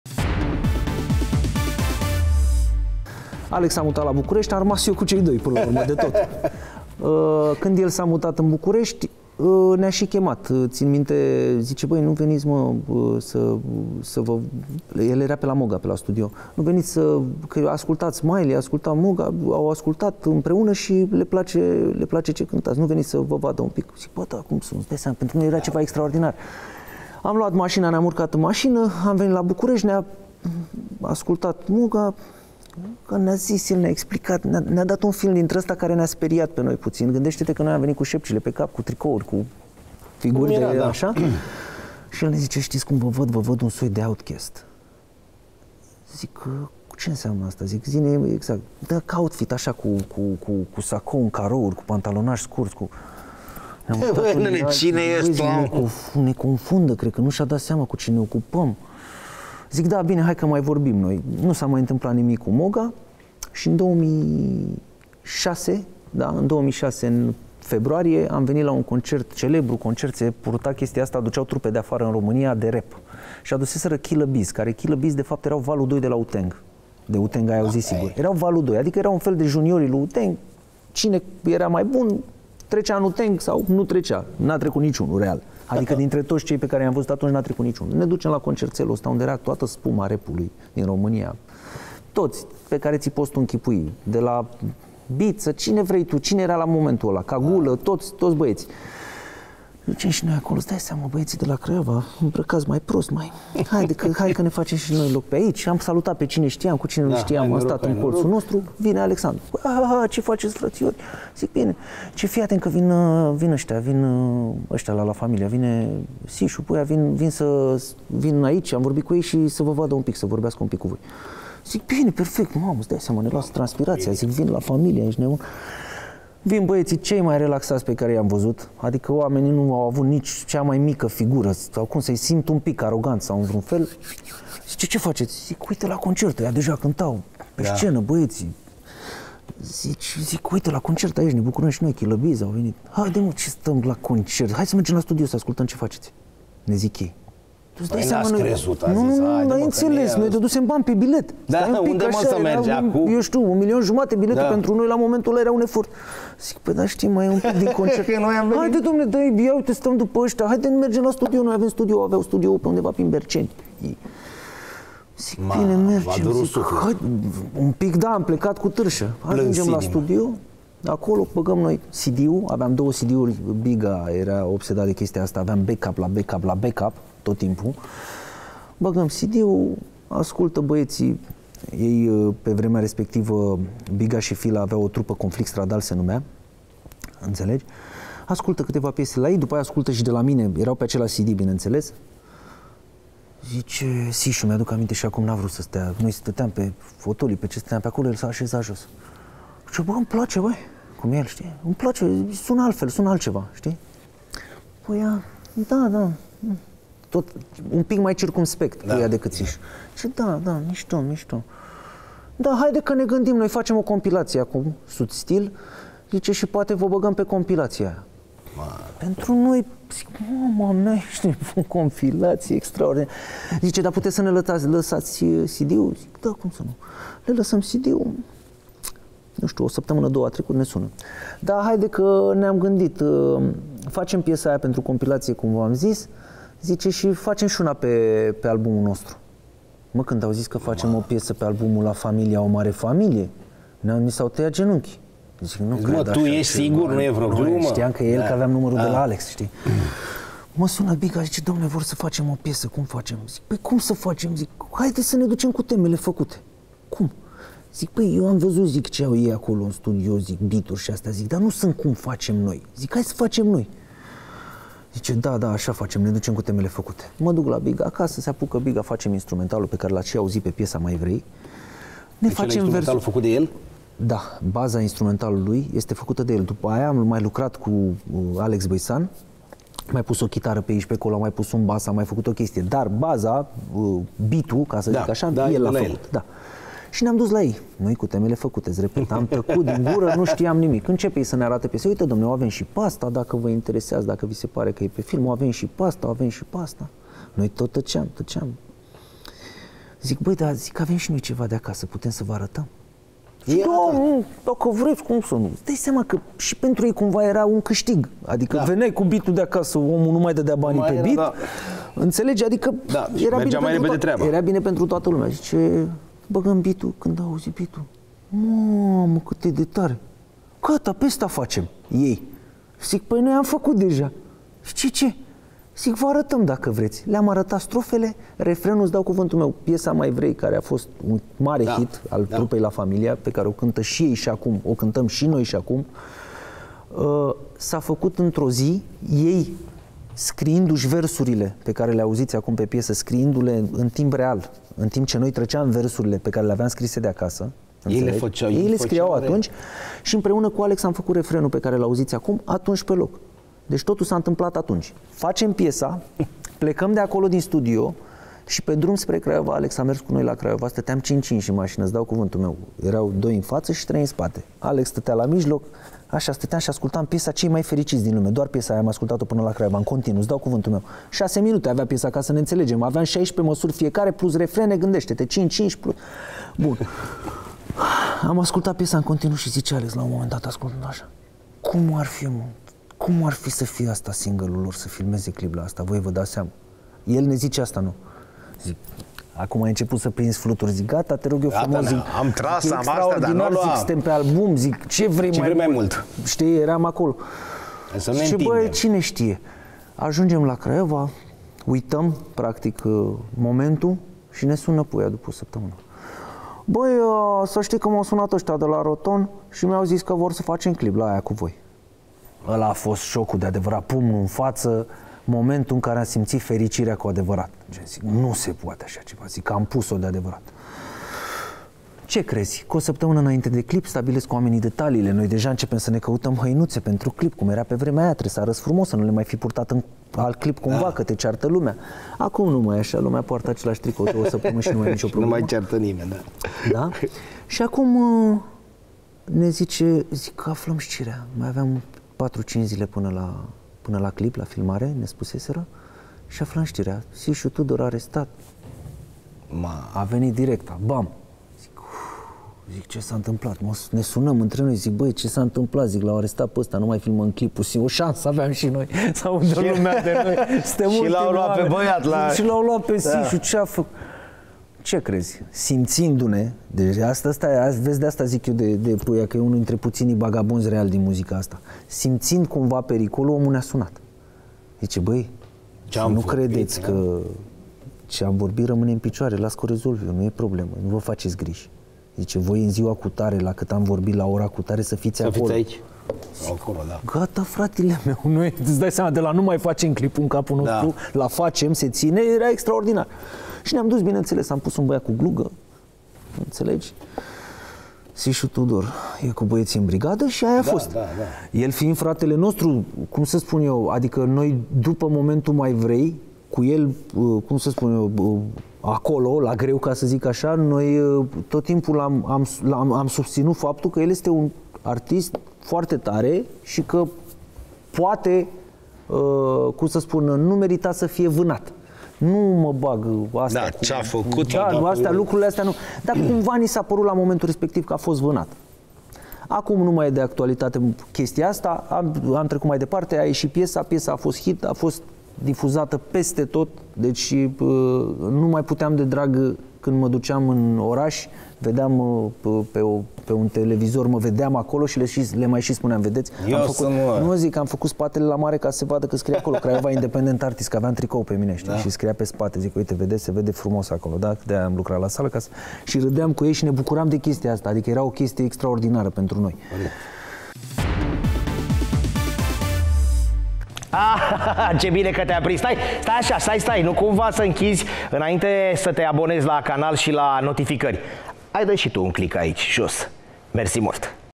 Rashid la AS.ro LIVE, partea a 4-a. Amintiri din rap-ul românesc din anii 2000
Rashid s-a născut în Craiova și este membru al trupei C.I.A. – Craiova Independent Artists. Primul contact cu muzică rap a fost cu Mc Hammer și Vanilla Ice.

În prezent, Rashid face parte din Golden Boy Society, proiectul care se bucură de un succes formidabil de câțiva ani buni, atât la nivel național, cât și peste hotare.